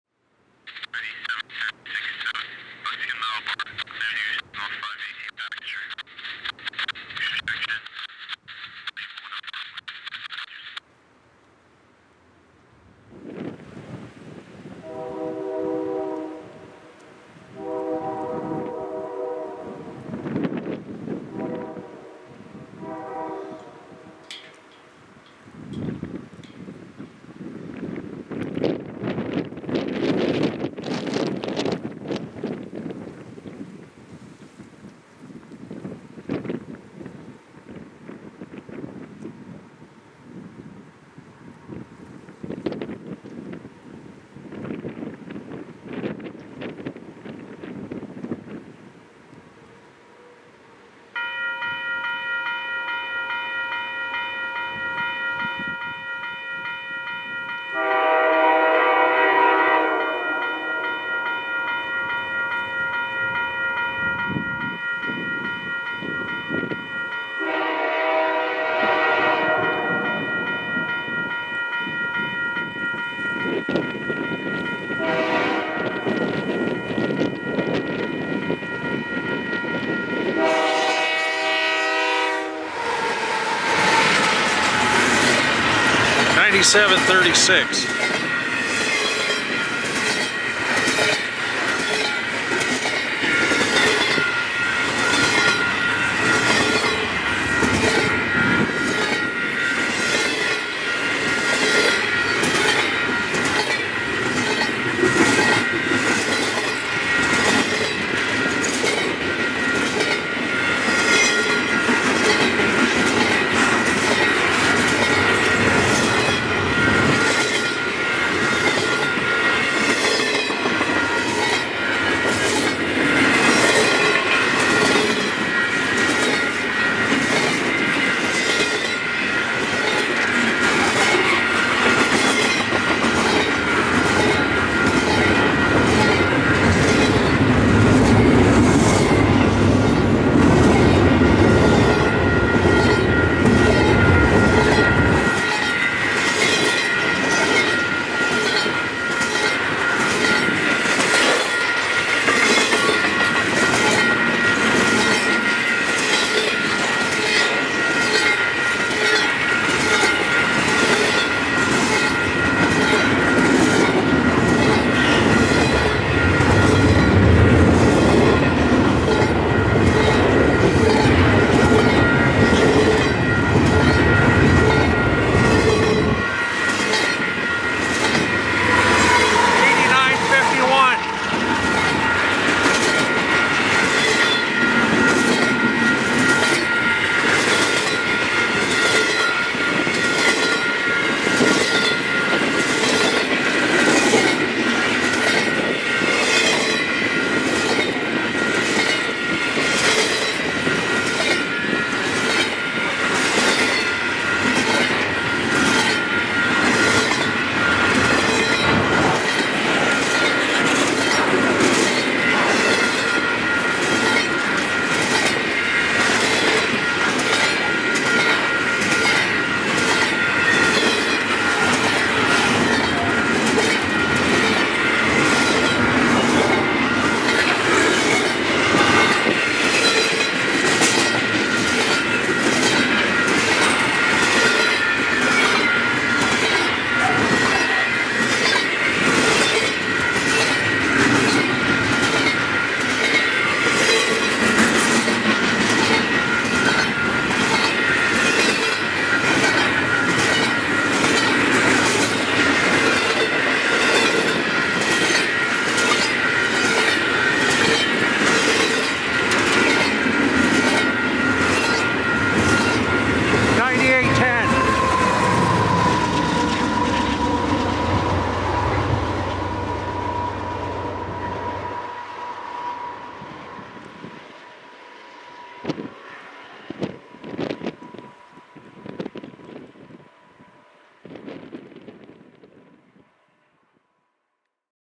CP 9736 at Buckskin (02FEB2023)
Canadian Pacific 9736, CP 8951, CP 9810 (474 axles) south passing Buckskin siding in Severn Falls, Ontario and cleared to 518 Baxter. No ground hog in their right mind would be out on a day like this! lol